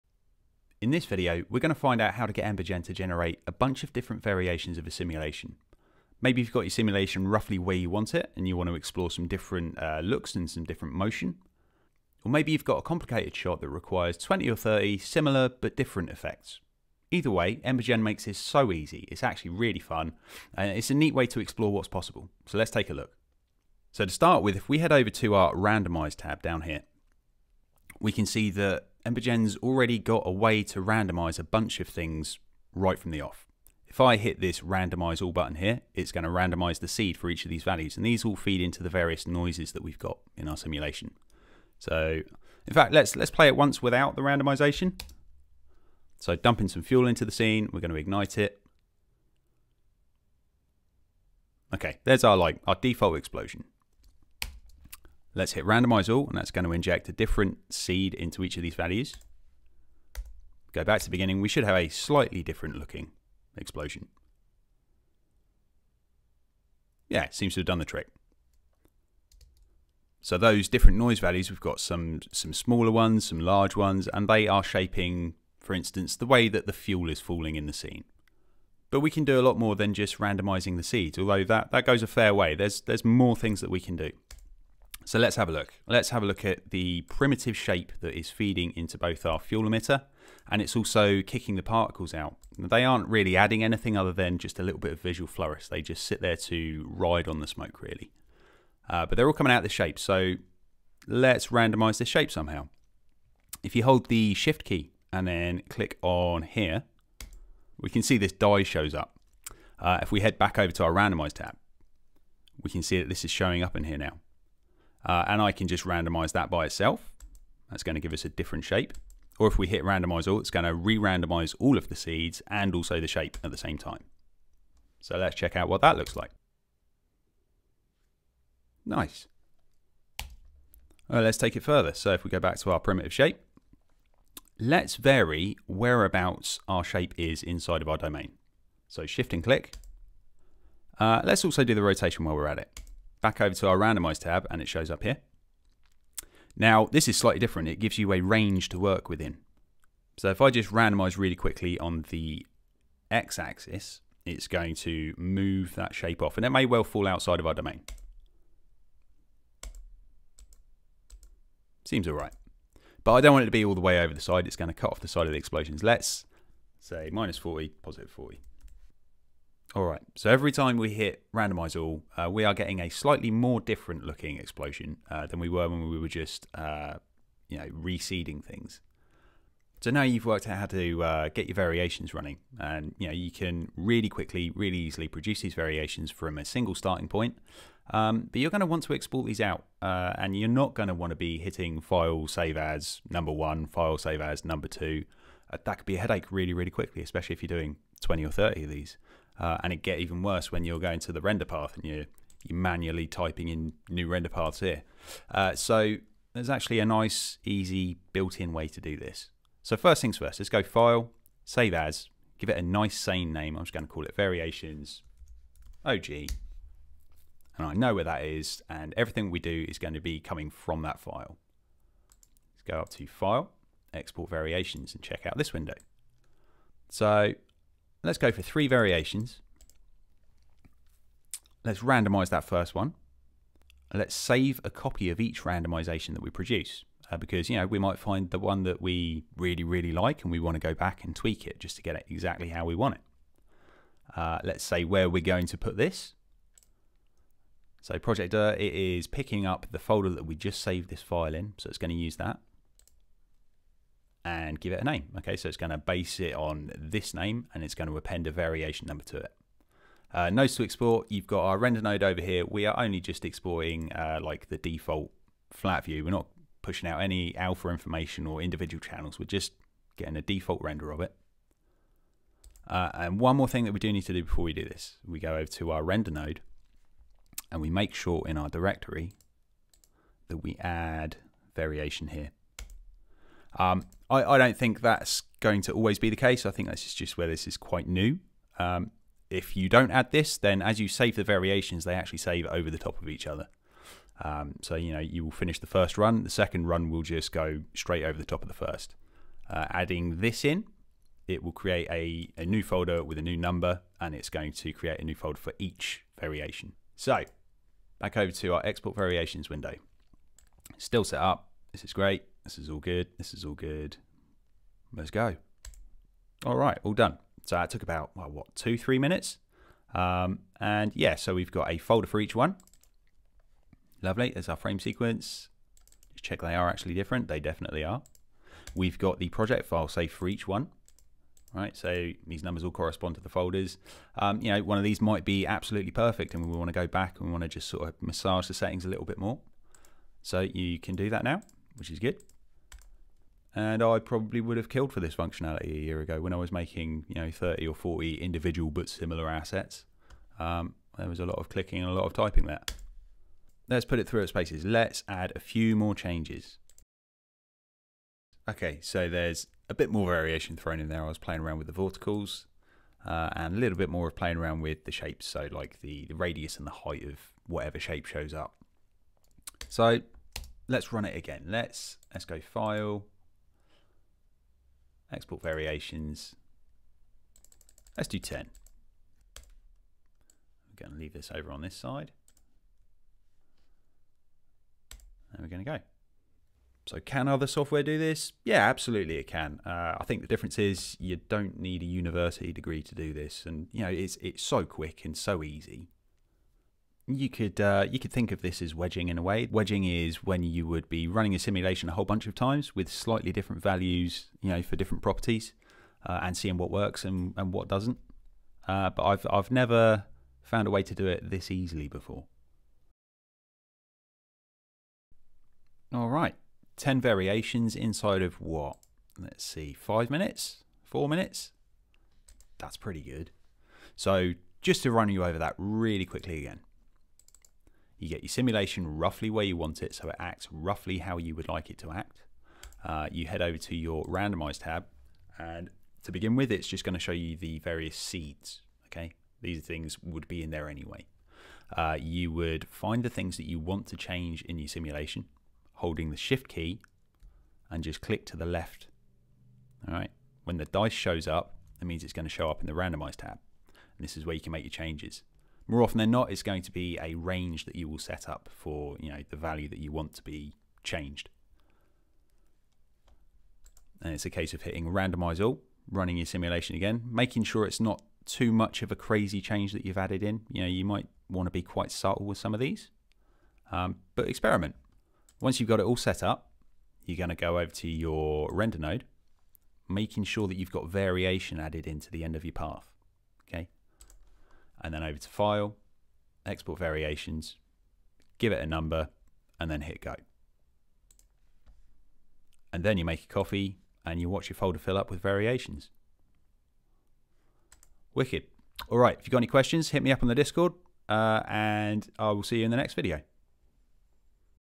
Quick EmberGen Tutorial: Creating Endless Variations - Batch Exporting
In this quick tutorial, we’ll take you through how to randomize parameters in your simulation, so that you can create new versions on-the-fly. We'll also show you how you can batch export as many different versions as you would like using the new Export Variations feature.

Here’s a link to the project file used in this tutorial

00:00 Introduction
00:23 Randomizing Seeds
01:18 Randomize Override
02:23 Randomizing Ranges
02:59 How To Export Variations
03:41 Export Variations Window
05:06 Let’s Export!
05:53 Let’s Export some more!
07:02 Recap